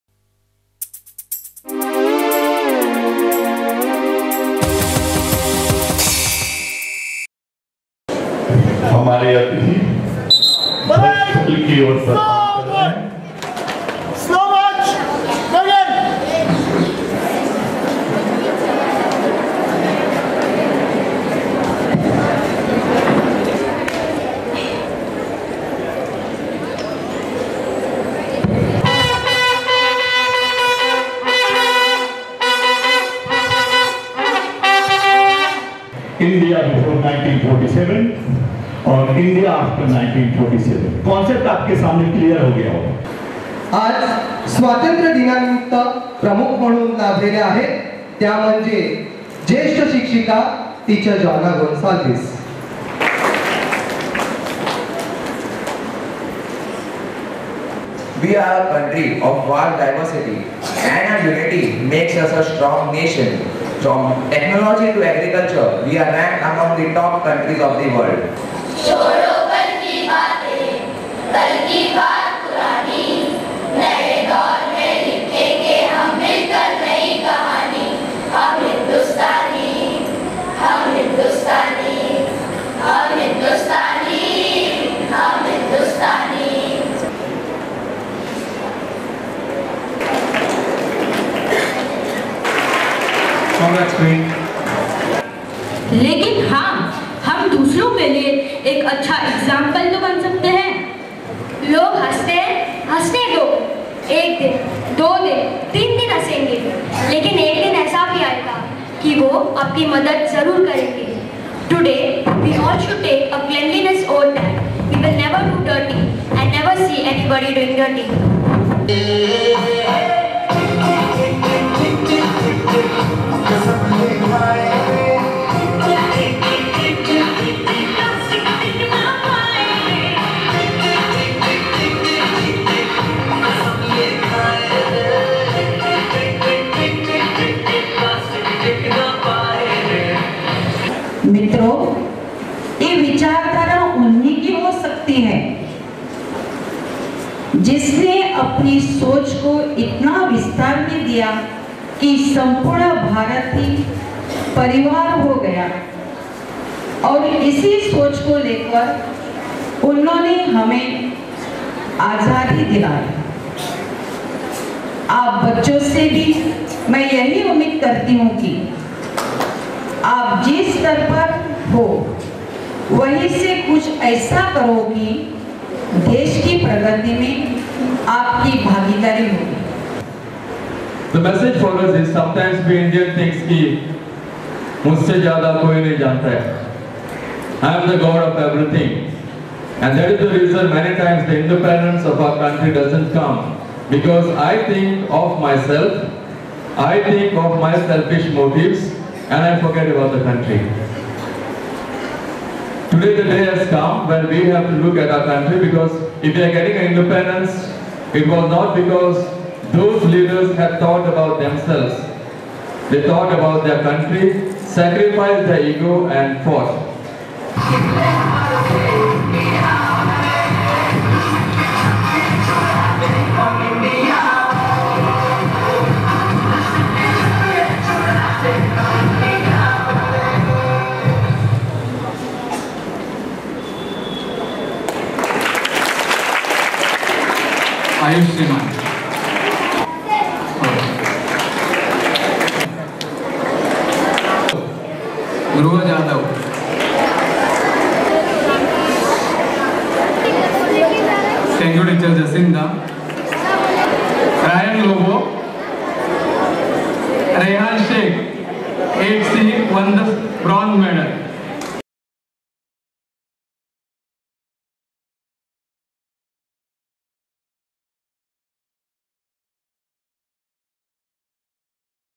FREEDOM EXPLORATION
Independence Day based on the theme "FREEDOM EXPLORATION" was celebrated by Theresians, Bandra.